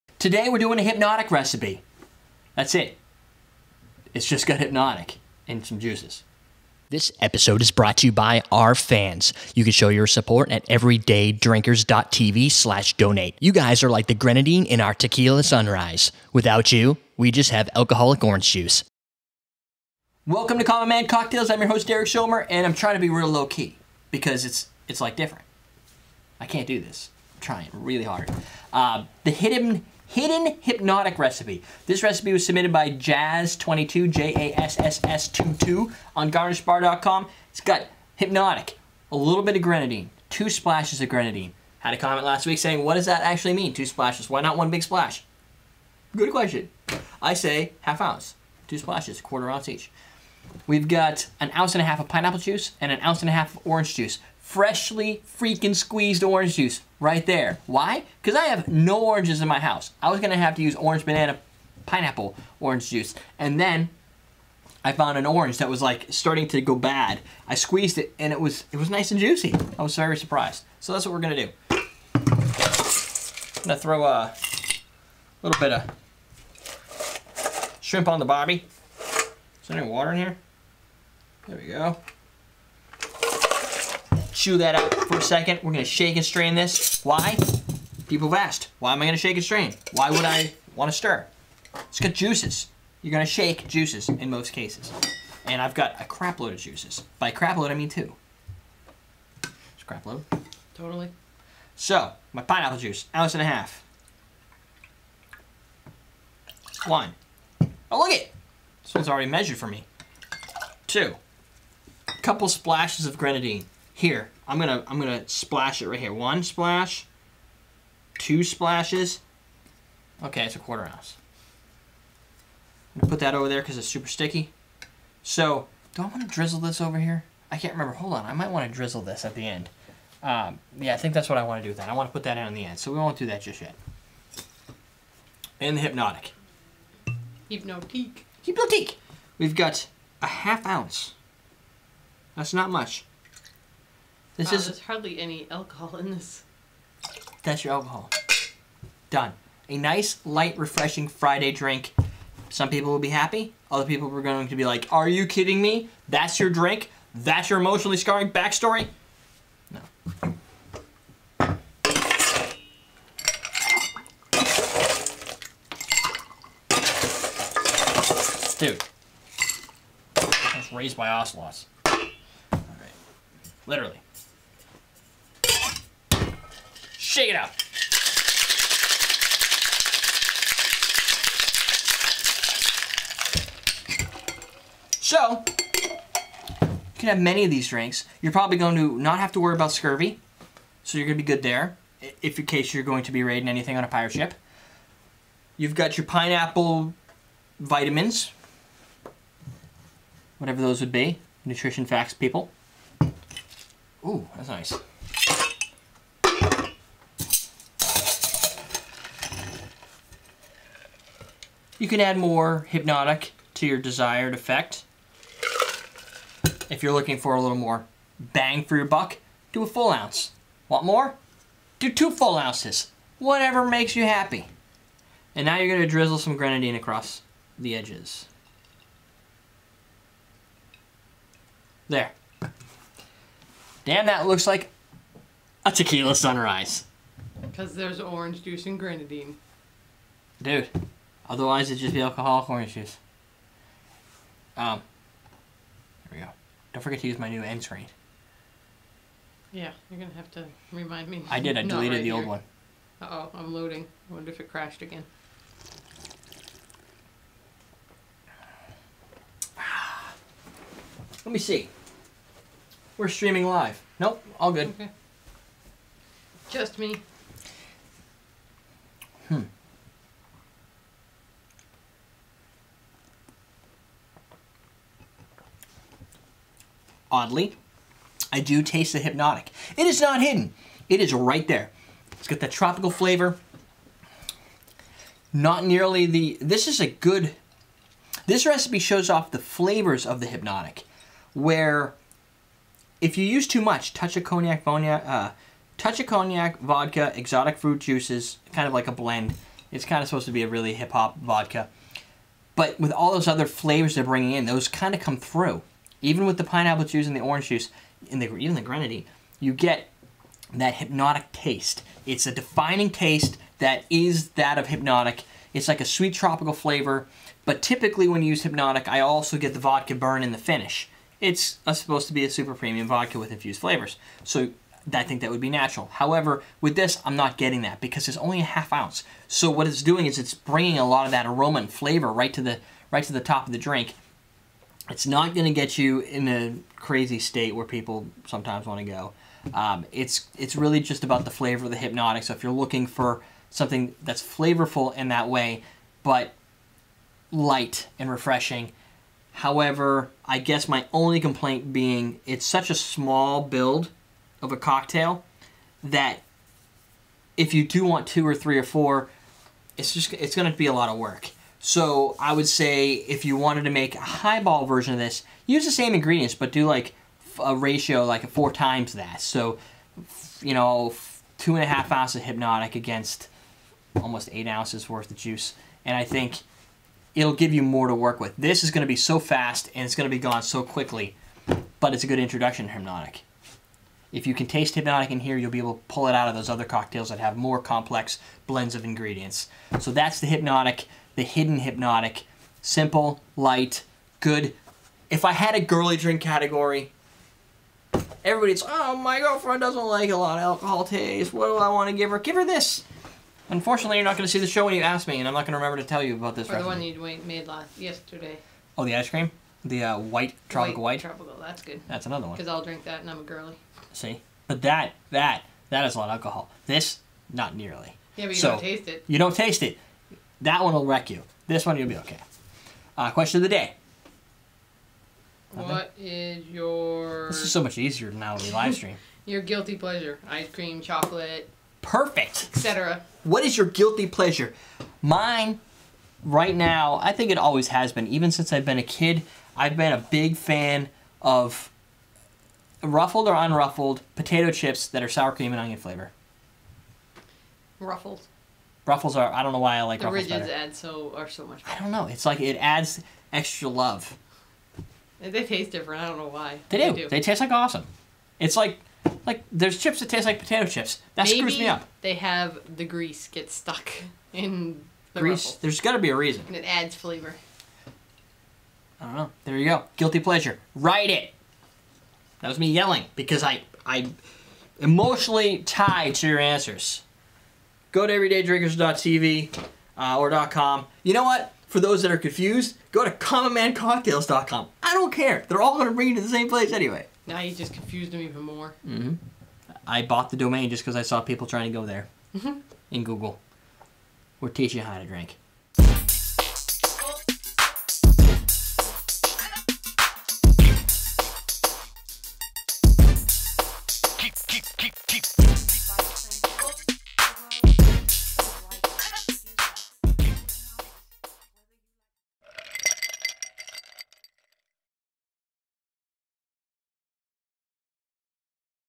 How To Make The Hidden Hpnotiq
This is a great cocktail for light weights! Recipe: 1/2 oz Hpnotiq, 1 1/2 oz Orange Juice, 1 1/2 oz Pineapple Juice, 2 Splashes Grenadine